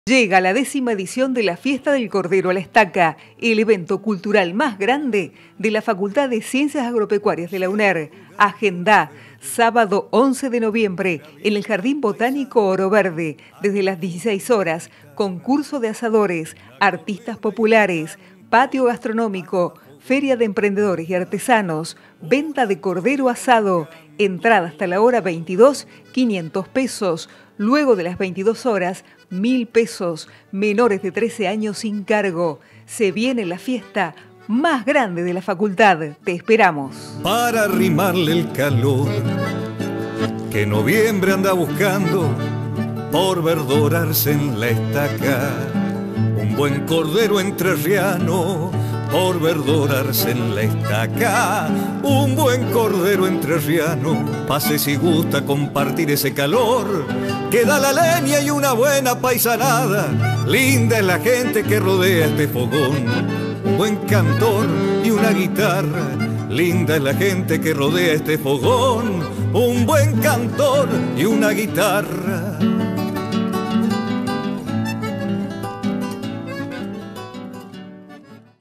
ORO VERDE - ENTRE RÍOS
📣10° Fiesta del Cordero a la Estaca - FCA UNER
 🎤💃🍽🐑👉 Agendá!! 11 de noviembre de 2023 - Desde las  16.00 hs.
✔Concursos de asadores.
✔Artistas populares.
✔Patio gastronómico.
✔Feria de emprendedores y artesanos.
✔Venta de cordero asado.
🙋‍♀️🙋‍♂️¿Te la vas a perder?
🔖ENTRADAS HASTA LAS 22 HS $500, DESPUES DE ESE HORARIO $1000. MENORES DE 13 AÑOS SIN CARGO